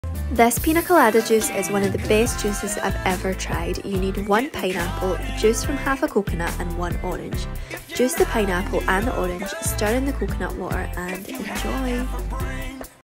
The Pina Colada juice recipe
This Pina Colada-inspired juice is one of the best juices I have ever tried 🍹 It is so tropical and yummy 😋 Recipe ➡️

- 1 pineapple 🍍
- Juice of 1/2 coconut 🥥
- 1 orange 🍊

1. Pierce the coconut and drain the juice into a glass.
2. Juice the pineapple and orange.
3. Mix in half of the coconut juice (save the other half for your next smoothie) and ENJOY!

A Piña Colada is a popular cocktail that typically consists of rum, coconut cream or coconut milk, and pineapple juice. It's often served blended with ice to create a refreshing and tropical drink. If you're looking for a non-alcoholic version or just a piña colada-flavored juice, you can create something similar without the alcohol. Here's a basic recipe for a Piña Colada juice:

Instructions:

Start by cutting up fresh pineapple into chunks. Make sure to remove the skin and core.

Place the pineapple chunks into a blender.

Add coconut milk or coconut water to the blender. Coconut milk will provide a creamier texture, while coconut water will be lighter and more hydrating.

If desired, add a sweetener like honey or sugar to taste. Remember that pineapple is naturally sweet, so you might not need much extra sweetener.

Blend the ingredients until smooth. If you prefer a slushier texture, you can also add a handful of ice cubes to the blender and blend again until the ice is crushed and incorporated.

Taste the juice and adjust the sweetness or coconut flavor as needed.

Once you're satisfied with the flavor and texture, pour the Piña Colada juice into glasses.

Taste the freshly squeezed orange juice. If you find it too tart, you can add a bit of sugar or sweetener to taste. Start with a small amount and adjust until you're satisfied.

Optionally, you can garnish the drinks with a slice of pineapple or a maraschino cherry.

Enjoy your homemade Piña Colada juice! It's a great way to capture the tropical flavors of the classic cocktail in a refreshing and non-alcoholic form.